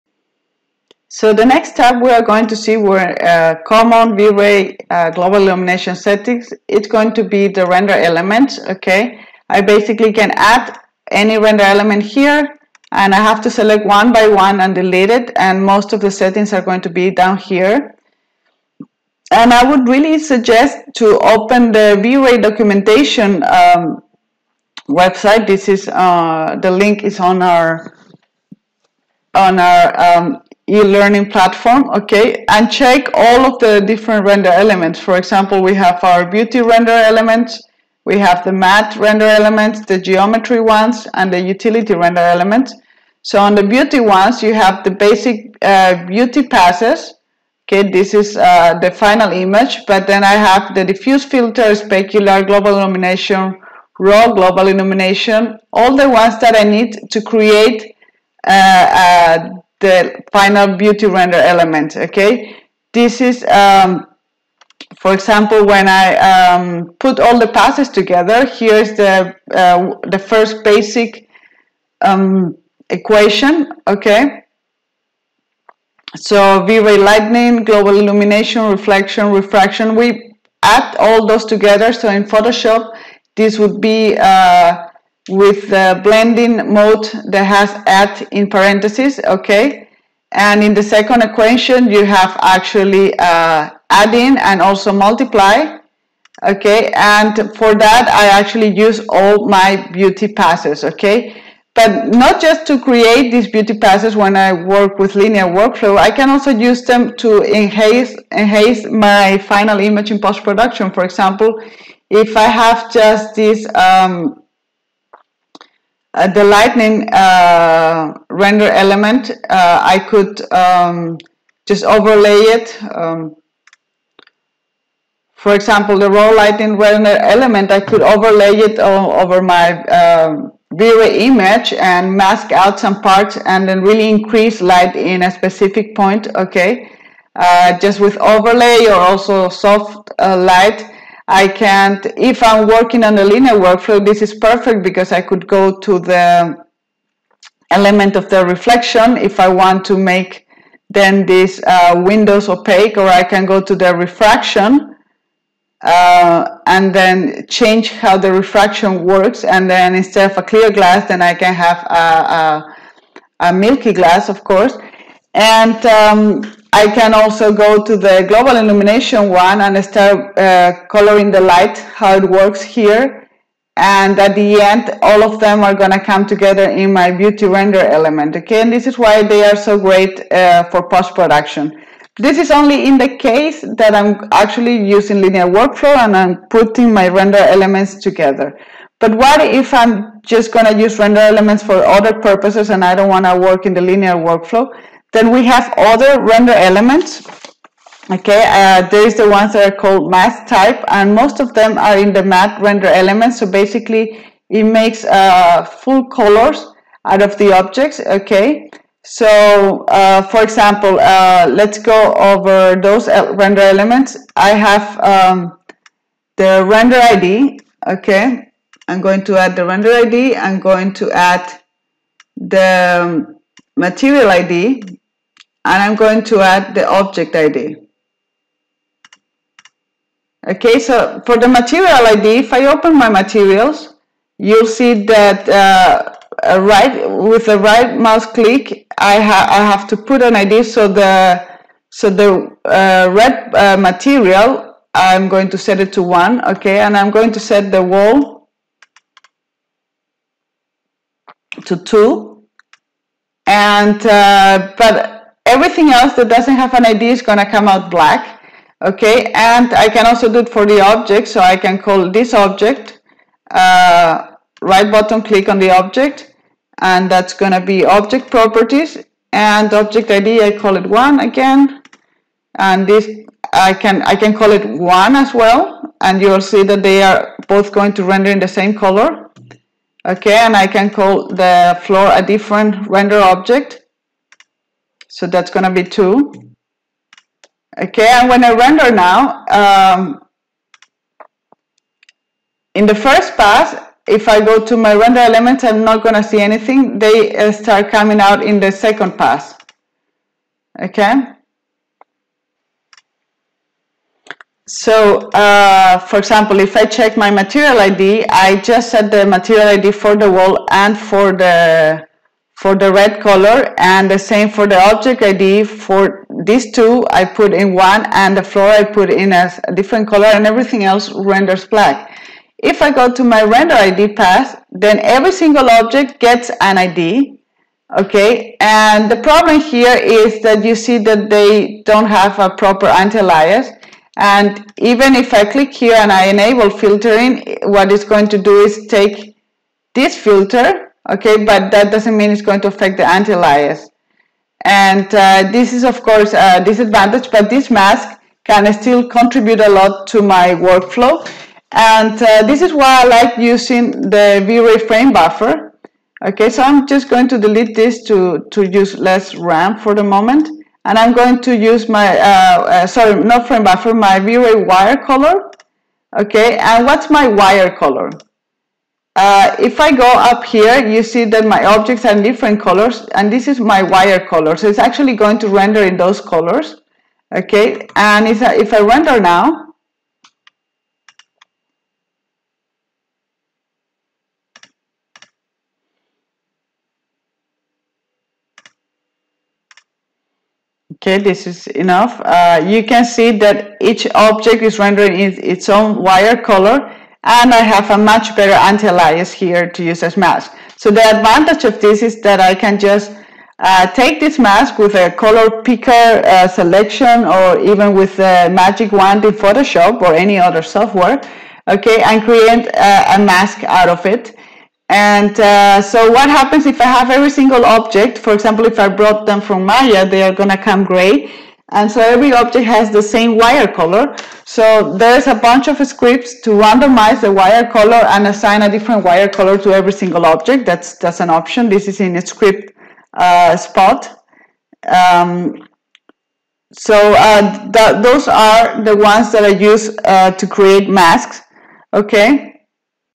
Render Elements: Beauty Passes and Matte elements
This is a series of short videos as part of a 3ds MAX Beginners course.
During the course we use VRAY as render engine

(Spanish) Los videos tienen subtitulos en español pero os recomiendo visitar 3d Collective o Adan Martín para aprender mas a fondo Vray y 3ds MAX.